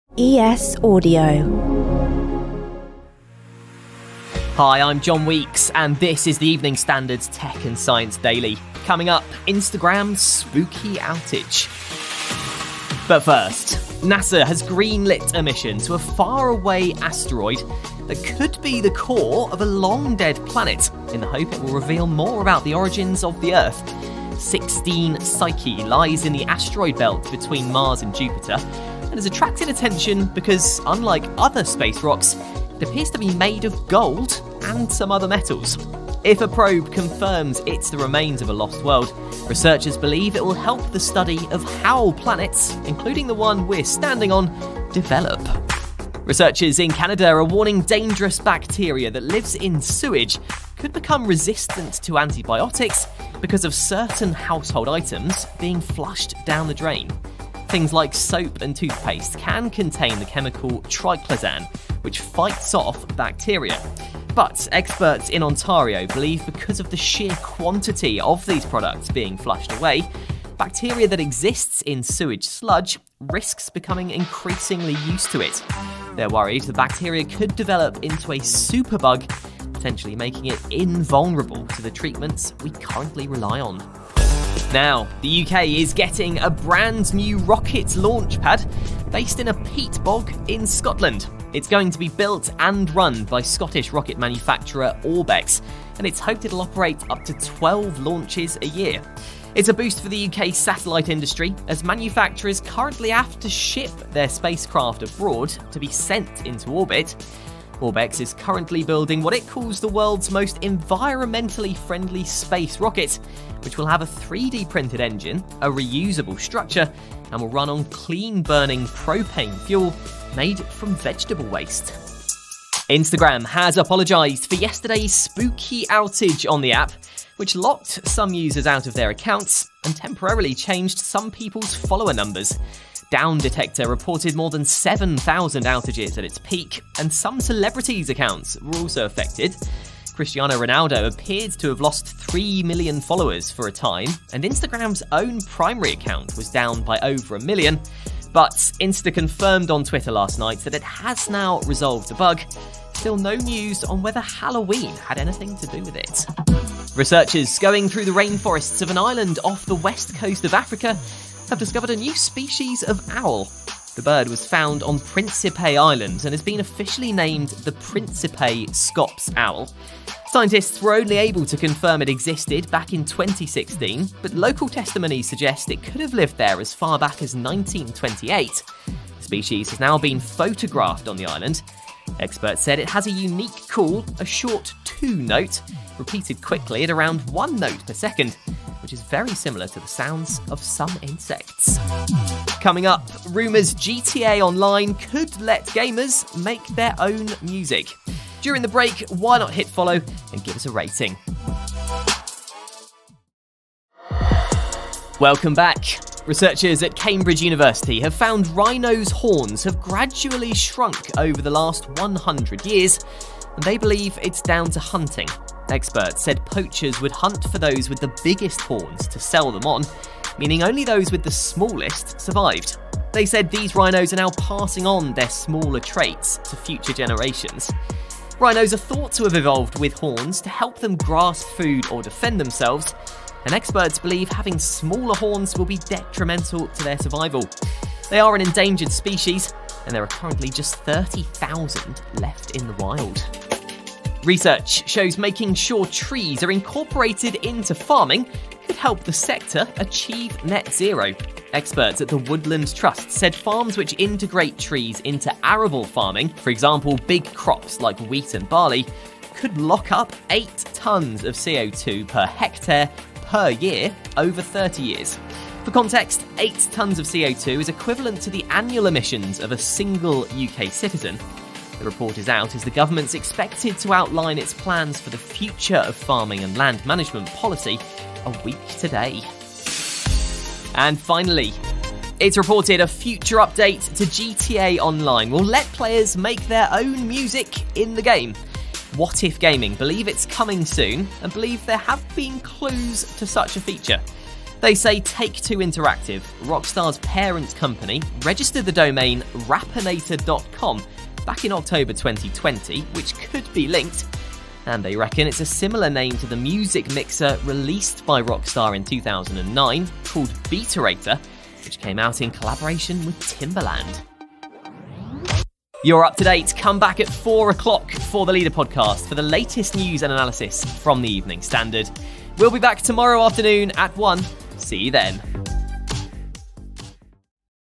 Nasa plans trip to 'golden asteroid' ...Tech & Science Daily
Nasa’s mission to a far-away ‘golden asteroid’. Could it be the core of a long-dead planet? Researchers in Canada issue ‘superbug’ warning about sewage bacteria. Instagram apologises for spooky Halloween outage on the app.
Plus:
The UK’s getting a brand new rocket launchpad
A newly discovered species of owl
Why Rhino’s horns have gradually shrunk over the last 100 years
Trees incorporated into farming could help the sector achieve net zero, study says
It’s reported a future update to GTA online will let players make their own music in the game